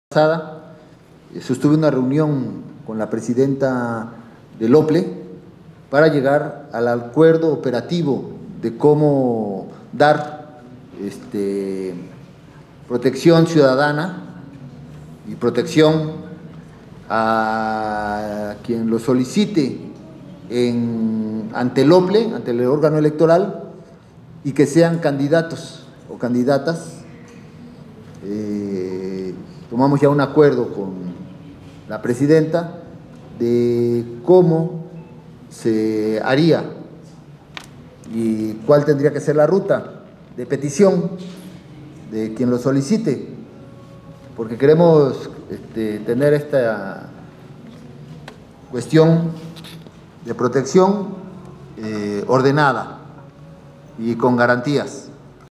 Protección inmediata a los candidatos que lo soliciten asegura Cuitláhuac García
Xalapa, Ver., 16 de abril 2024.- Después de que hoy el presidente de la república Andrés Manuel López Obrador, en su mañanera anunciara que se dará protección a cualquier candidato que lo solicite; hoy el gobernador de Veracruz, Cuitláhuac García Jiménez, hizo lo propio.

📽️ Luis Sigüenza